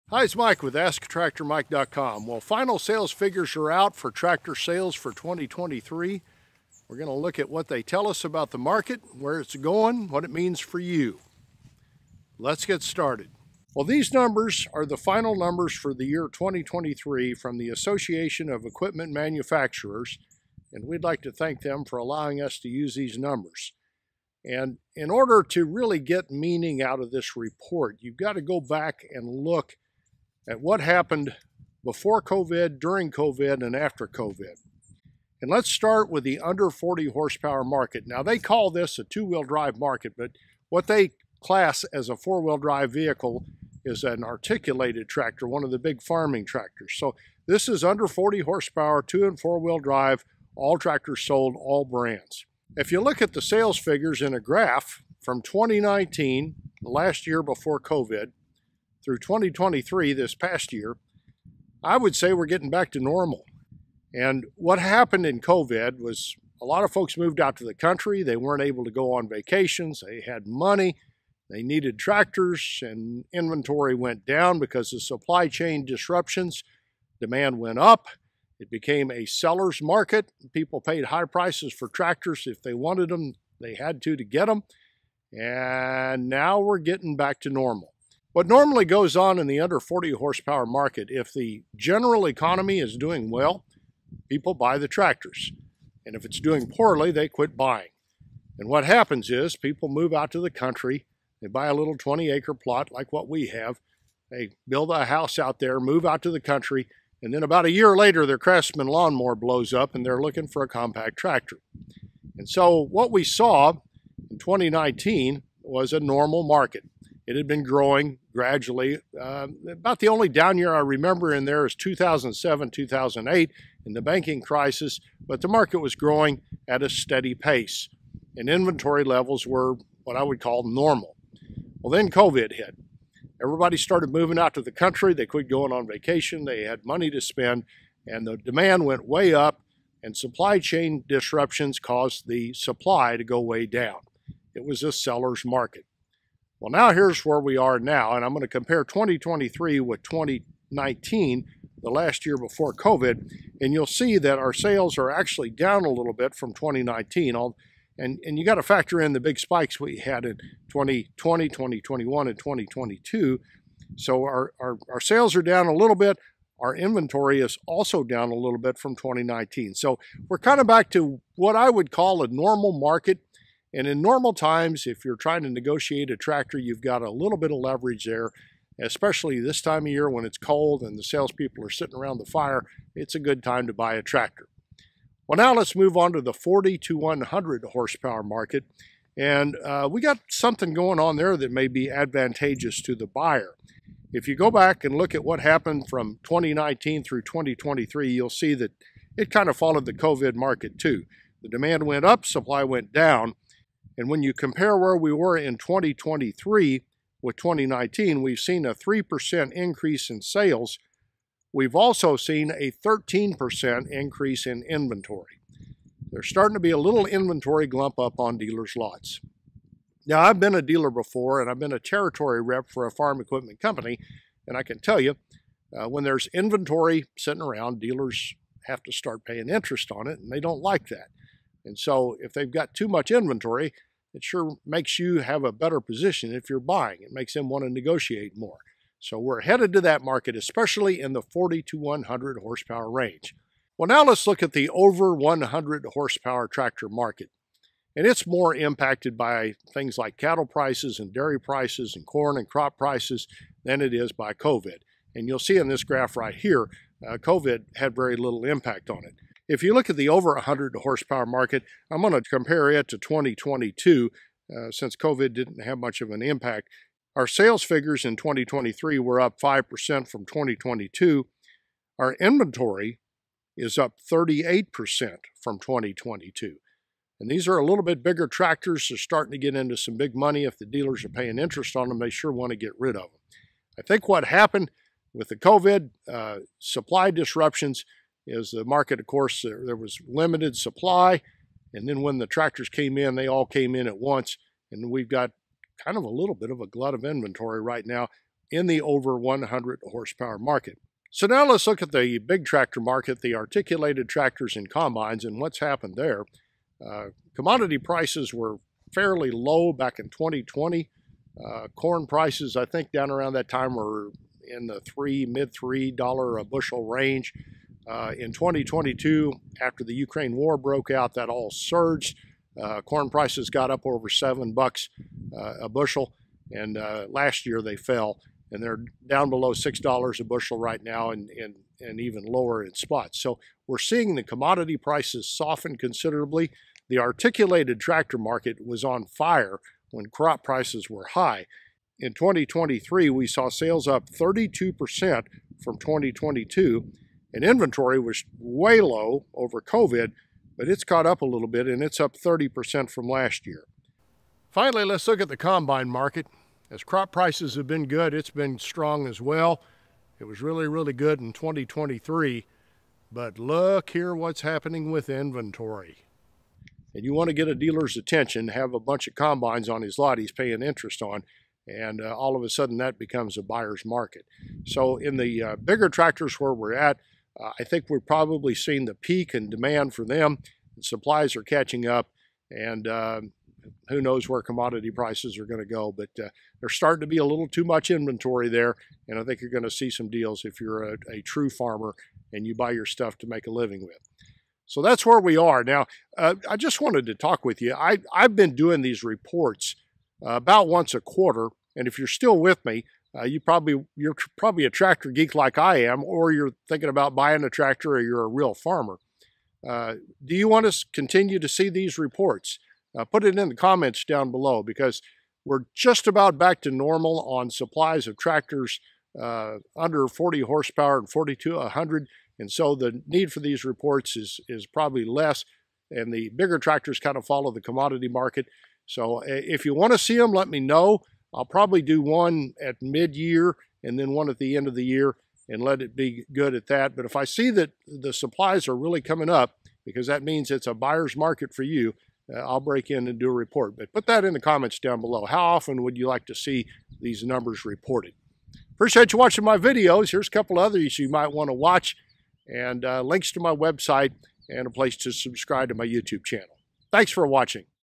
Breaking Down 2023 Tractor Sales Numbers: Deals & Dips in the Agriculture Market
Dive deep into the latest data as we decode the final 2023 tractor sales numbers released by the Association of Equipment Manufacturers! 🚜📊 In this comprehensive video, we navigate through the nuances of the agricultural machinery market, shedding light on key insights that could shape your decisions in the coming months.

The statistics reveal a compelling narrative, especially for tractors under 40 hp, experiencing a notable 10.7% dip in sales from the previous year. Examining the context, we explore the aftermath of the supply chain disruptions of 2022 and draw comparisons to the pre-COVID year of 2019, unraveling shifts in demand and supply. Witness the evolution from 163,079 compact tractors sold in 2019 to 156,070 in 2023, with inventory concerns beginning at 92,987 units.

Venturing into the 40-100 hp tractor market, we uncover a 9.1% decrease in sales from 2022, contrasting it with the positive trajectory seen in 2019. Mid-range tractors show resilience with a rise in sales, but inventory pressures are evolving, standing at 40,176 units in 2023 compared to 35,708 in 2019.

The realm of tractors over 100 hp paints a different picture, marking a 5.2% surge in sales for 2023. Articulated tractors skyrocket by 31.7%, and combines experience a modest 1.7% uptick. However, as we dissect the intricacies of the crop production sector, we witness brisk sales and low inventories, coupled with a warning sign – a significant decline in sales as crop prices slump. Could 2023 be the zenith in large tractor and combine sales?

But how does this intricate web of statistics impact you, our viewers? If you're a customer for a small or mid-range tractors, the time is now to seize every opportunity as we approach a potential buyer's market. Learn why negotiating with dealers could be more fruitful than ever, especially during the historically slow periods of January and February. The looming spring rush could pose challenges for dealers if they fail to proactively manage inventory, potentially leading to aging stock.

Join us as we navigate through these market dynamics, providing insights and strategies for both buyers and dealers in the ever-evolving landscape of agricultural machinery. Don't miss out on the chance to stay ahead in the world of tractors, where opportunities and challenges coalesce. 🌾💼

LINKS YOU NEED TO KNOW ABOUT...

00:00 Introduction
00:35 Compact tractor sales
03:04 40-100 hp tractor sales
04:03 Tractors over 100 hp
05:34 4wd Tractors & Combines